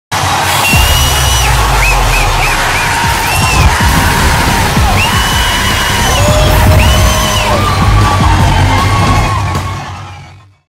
Who Else Want To Experience Your Fav Tv Show in Theater Like This ? 😂😂😂
Ps : Made it using after effects 😜
Footage of fans going crazy @ Rohini Silver Screens